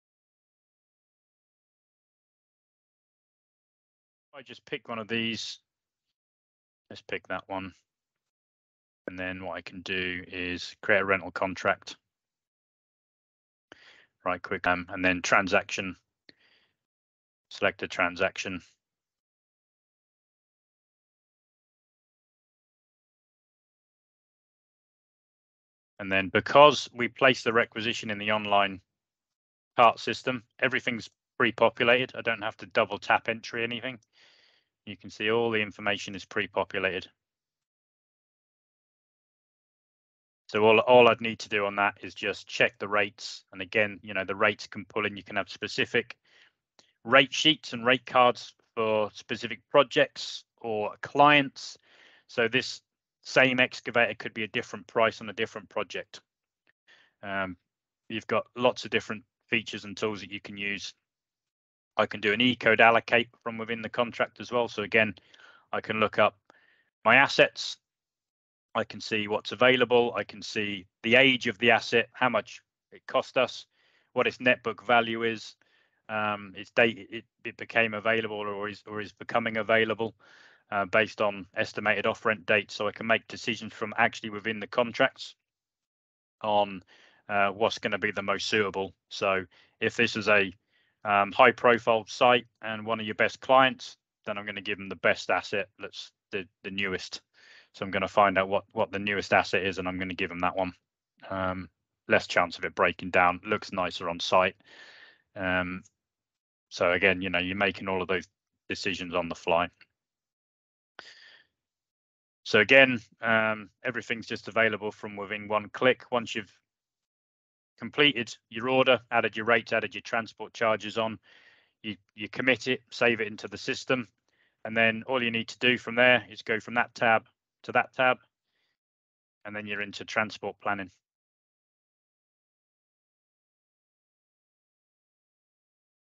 Efficient Equipment Rental: Streamlining Transactions with Advanced Online Systems
Dive into the world of hassle-free equipment rental with our customer portal that revolutionizes the way rental contracts are created and managed. In this video, we demonstrate the ease of selecting an asset and generating a complete rental contract swiftly. Watch as we navigate the online portal, showcasing how the system auto-populates all necessary information, eliminating the need for repetitive data entry.

Learn how to check and adjust rates according to specific projects or clients, ensuring tailored pricing with ease. Explore the advanced features that allow for e-code allocation and asset selection based on criteria such as age, cost, net book value, and availability. Gain insights into strategic decision-making for high-profile clients by allocating the newest and most reliable assets. Finally, we wrap up with a seamless transition into transport planning, all within a few clicks. Join us to discover how to enhance your rental process efficiency and impress your clients with top-notch service.